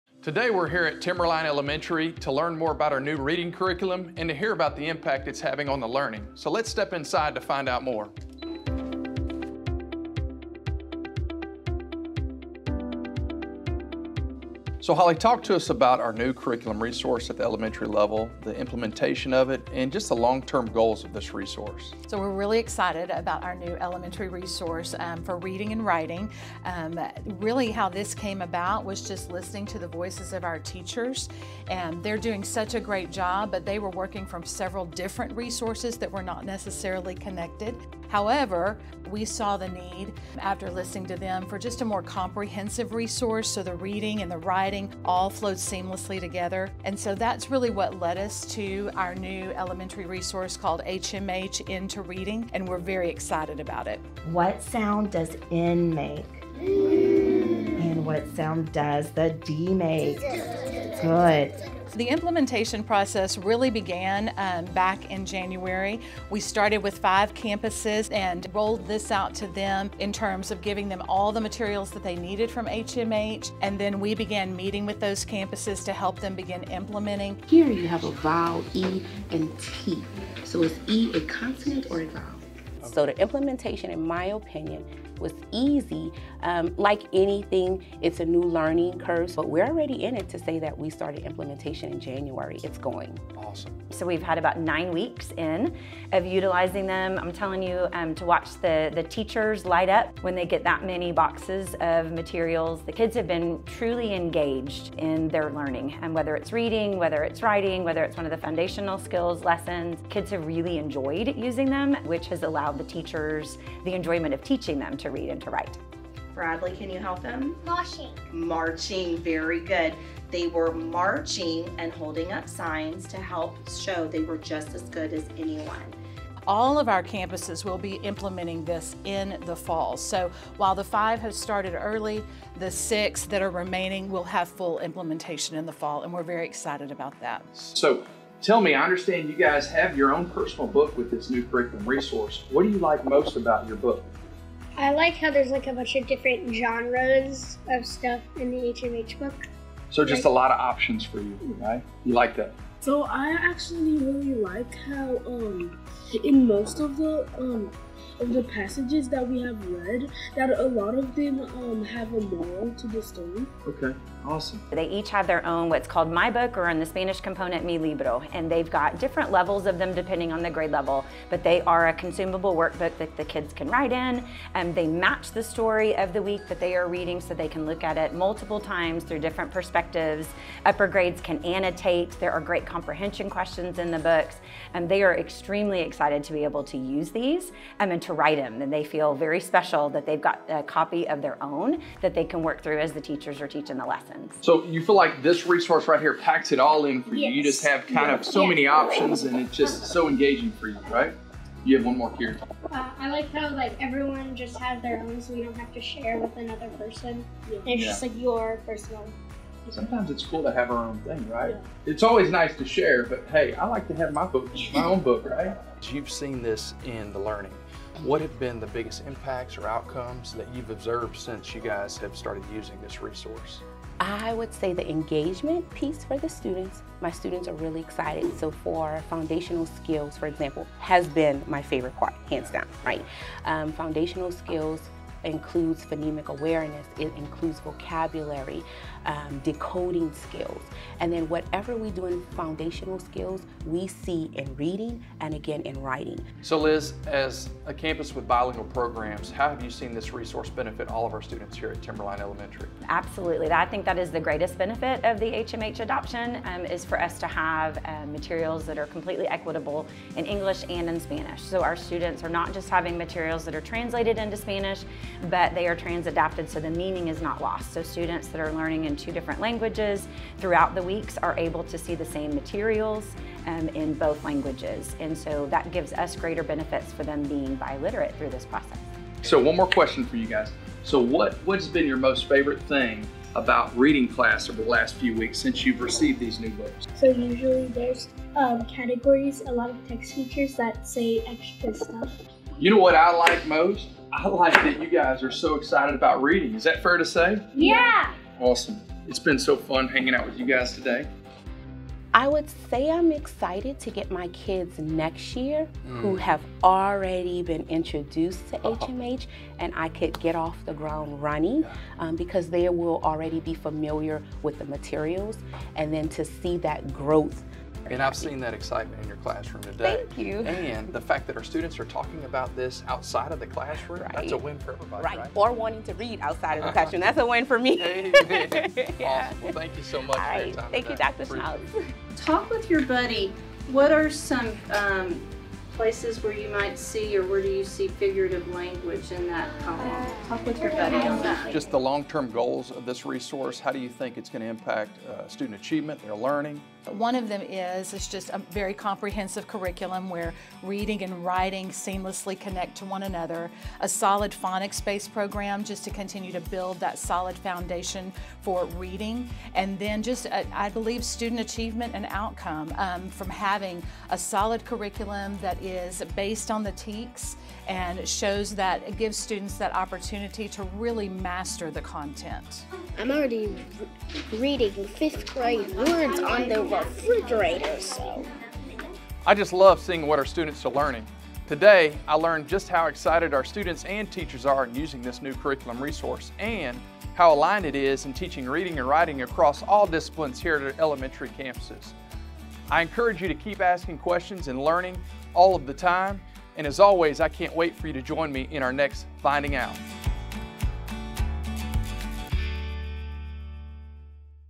Finding Out with Dr. Schnautz: New Reading Curriculum Resources
Join Dr. Schnautz as he visits Timberline Elementary to find out more from our teachers, students, and campus leaders about the new reading curriculum resources and the positive impact they are having on our students and their learning.